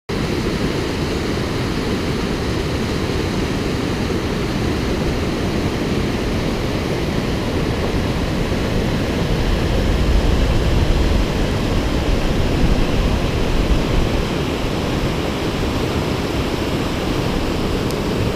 Oberaargletscher panorama view
Panoramic view of the Upper Aare Glacier (in german Oberaargletscher) in Central Alps, near to  Grimsel Pass.